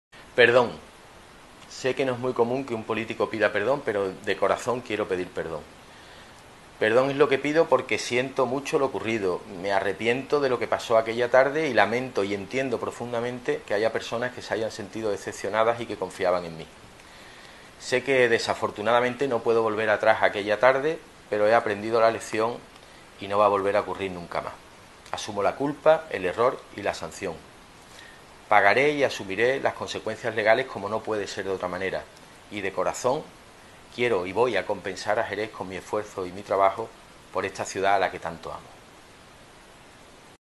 Saldaña publica un vídeo pidiendo perdón: "Lo compensaré con mi trabajo"
El líder del PP de Jerez dice que "asumo la culpa y la sanción" por lo ocurrido. "He aprendido la lección"